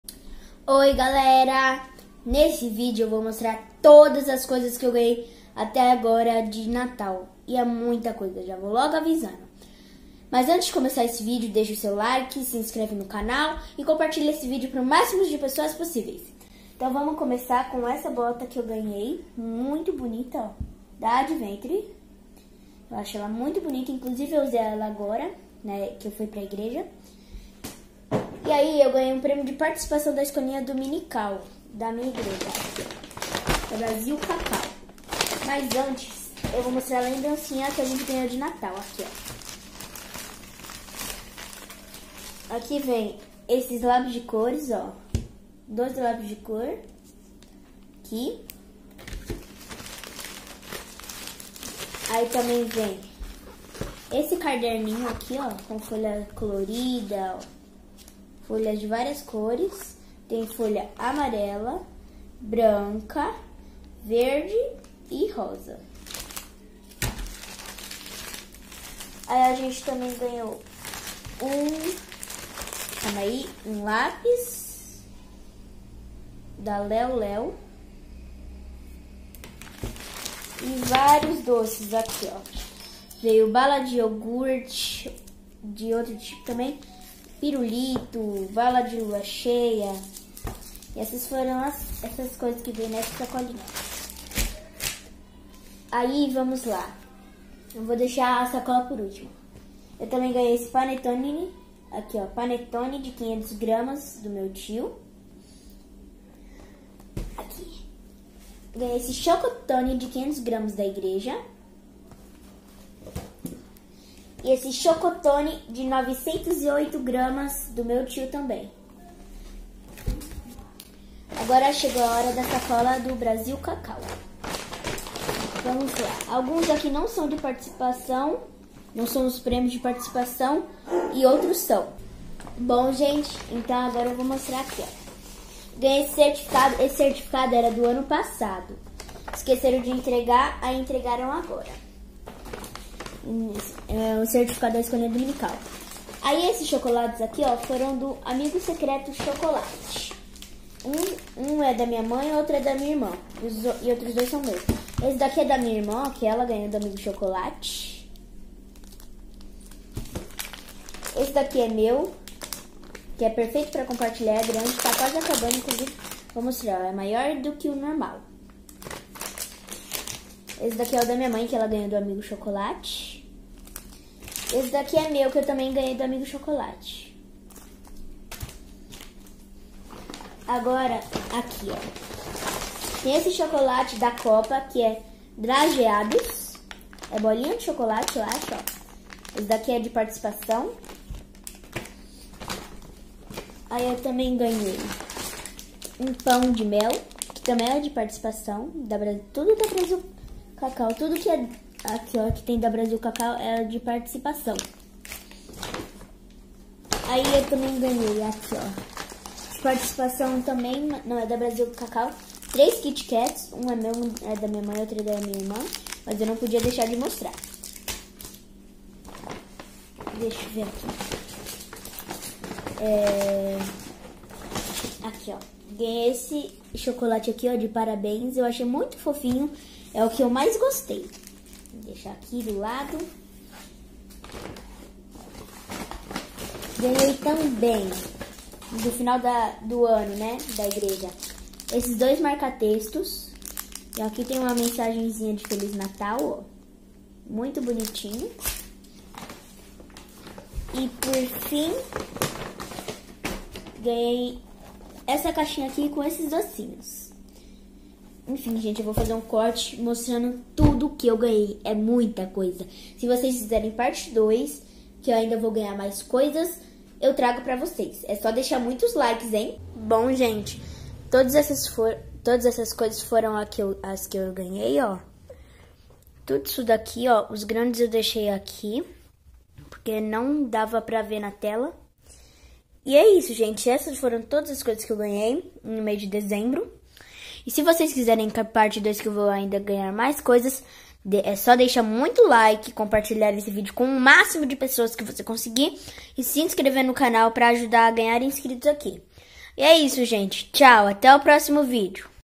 Mostrando as Coisas que Ganhei em Dezembro
Nesse vídeo, João Victor Kids Mostra todas as coisas que ganhou no mês de dezembro